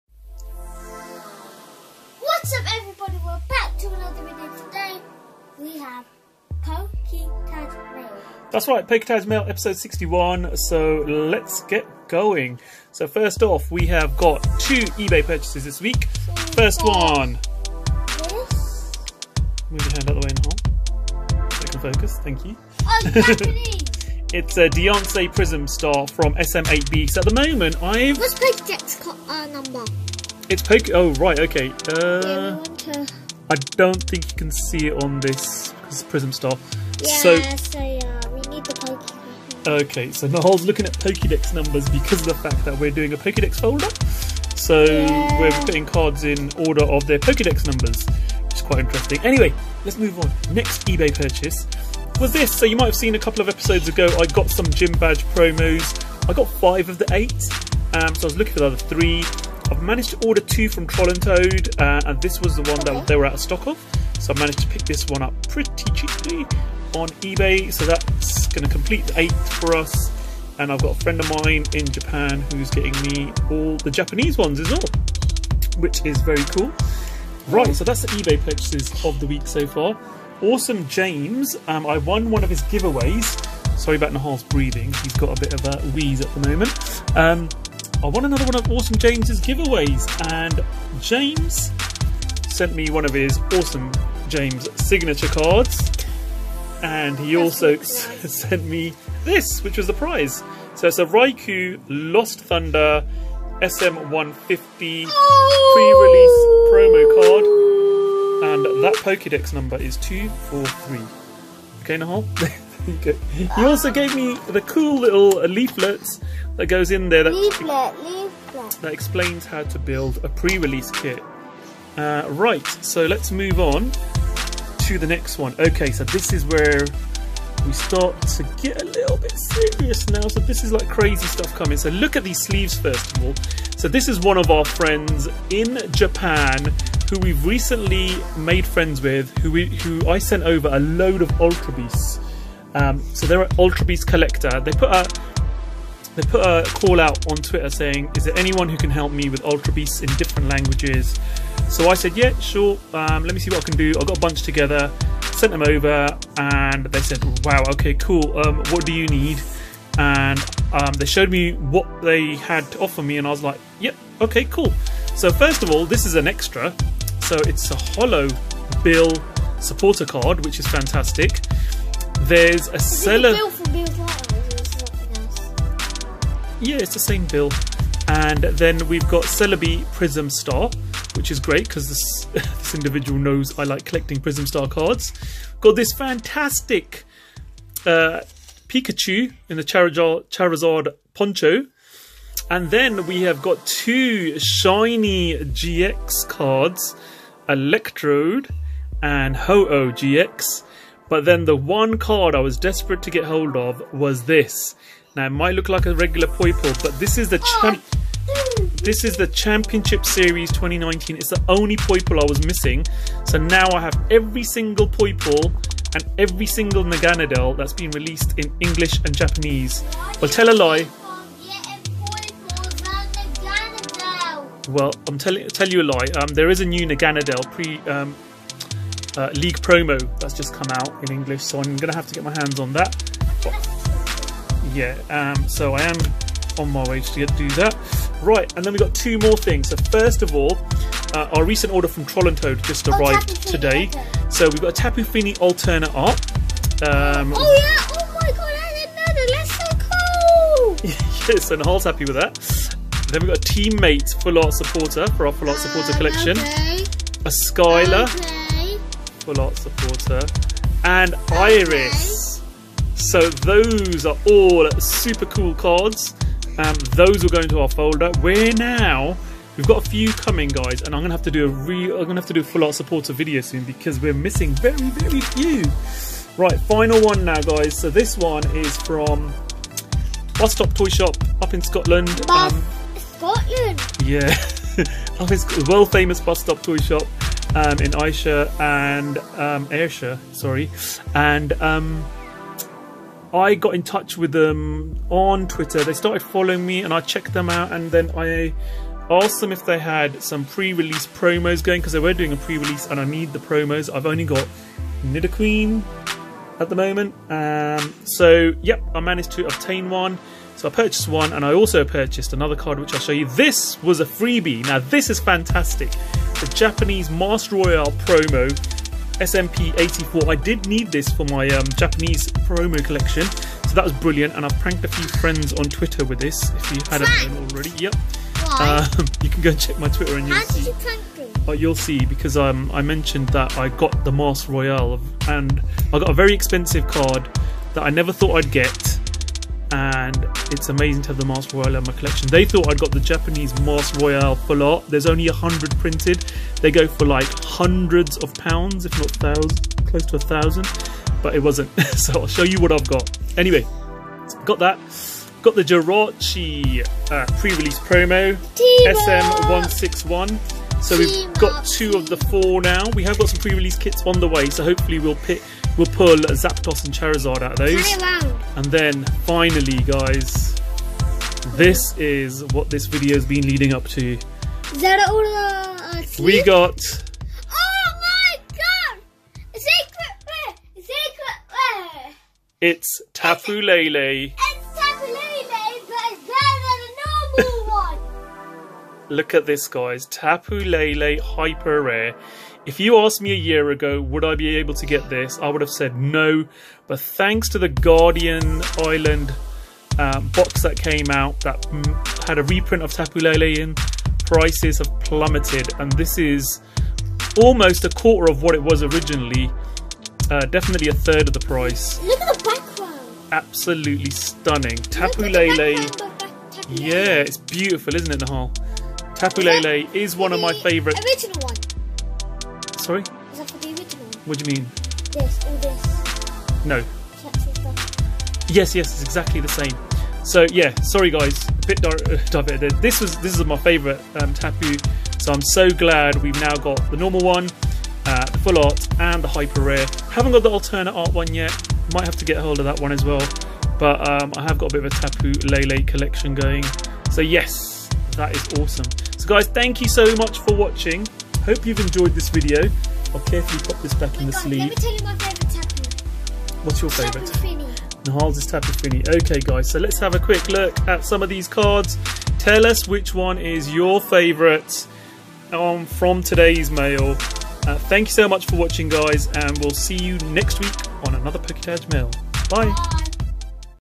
Pokémon PokéTaj Mail Episode 61 - The Mother of All Rainbow Rares!!!
In this video we open our weekly mail. We start with some ebay purchases, then move onto a great giveaway win from Awesum James, a fantastic first trade with our new Twitter friend in Japan @Anabel_lilac, our latest Troll and Toad order and finally an amazing card from Bus Stop Toy Shop in Scotland (Twitter: @busstoptoyshop

Follow us on:
Pokemon TCG Online: TajGooner